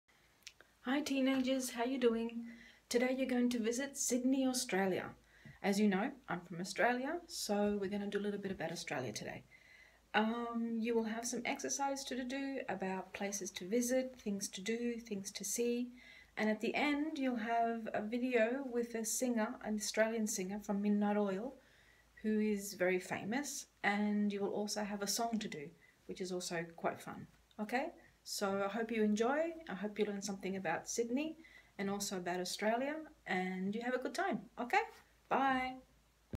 SYDNEY
For teenagers. Let's go to Sydney (Australia)! By Ingrid